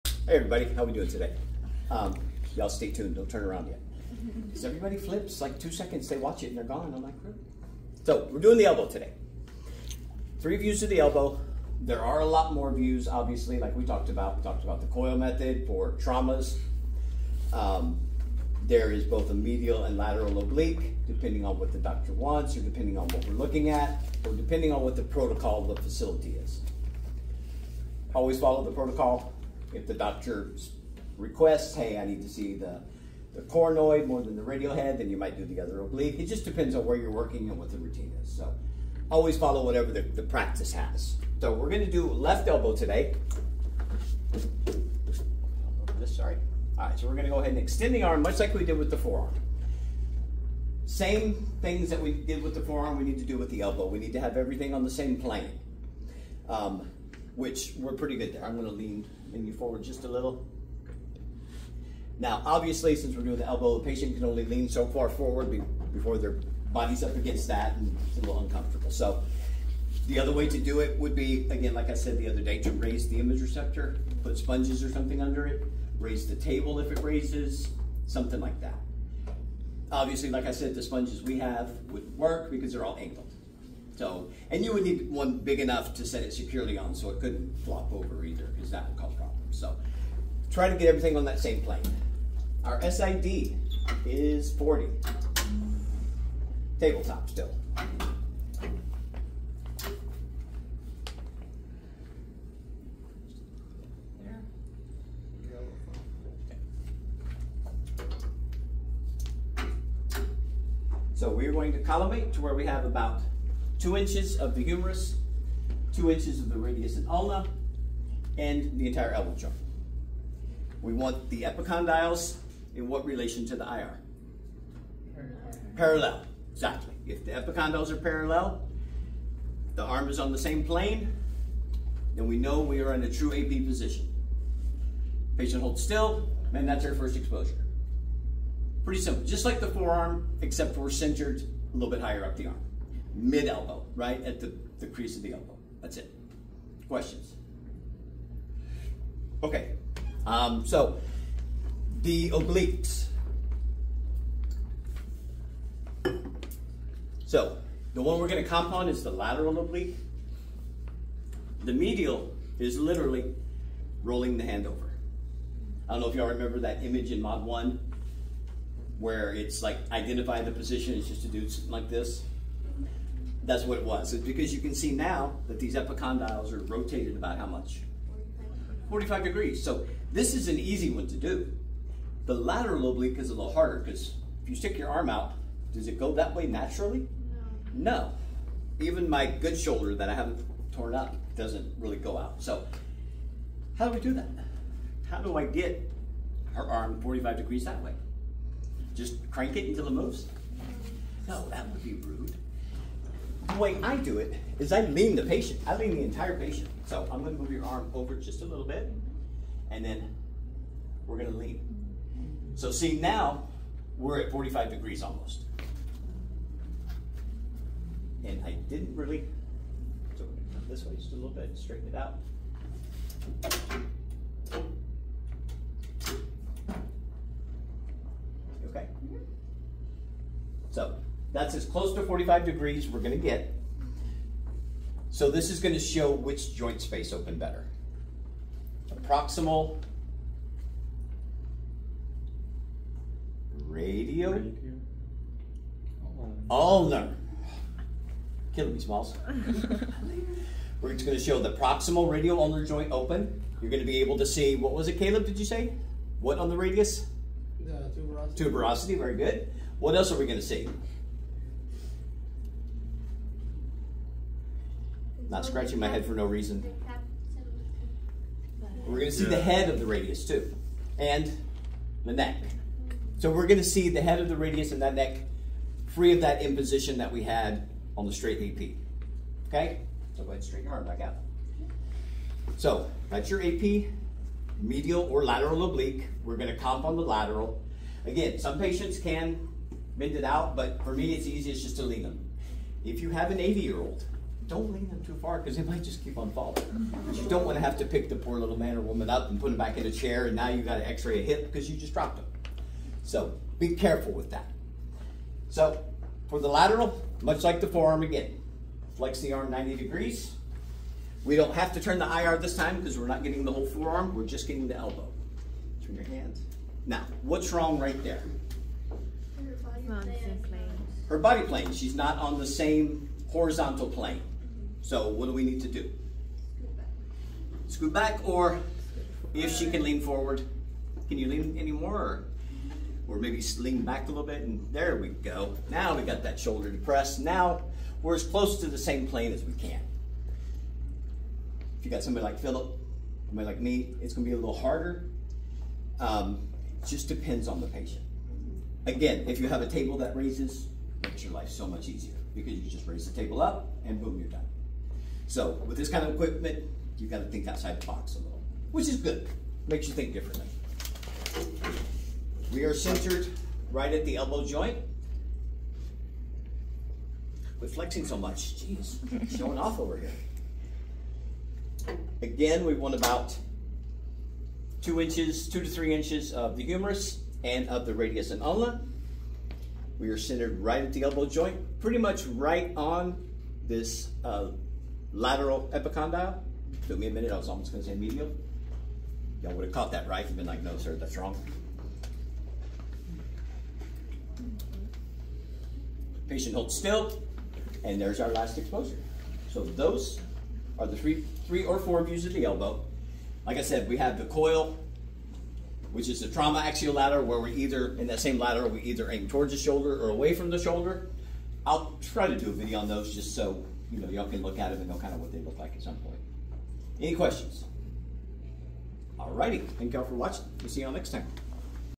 Elbow X ray positioning routine.
This is the AP. oblique, and lateral projections of thr elbow.